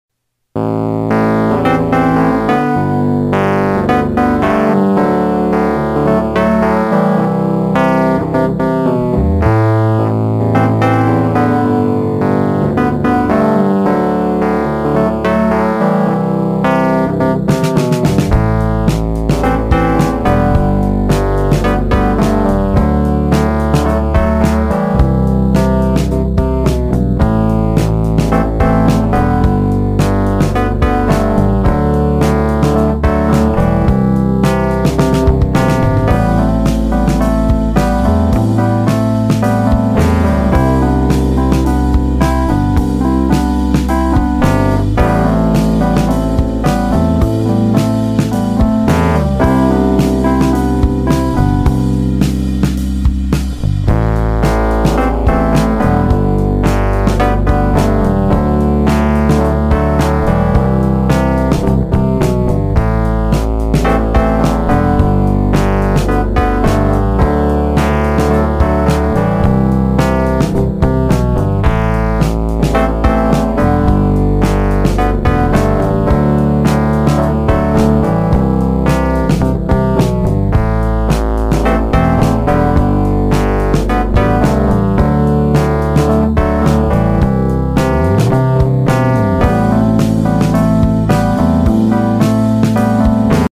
Mother nature takes a dump on my neighborhood
When Mother Nature hates your neighborhood she rains on it so much that the sewer starts to back up and the roads flood😆 Luckily there was no damage to the houses !!!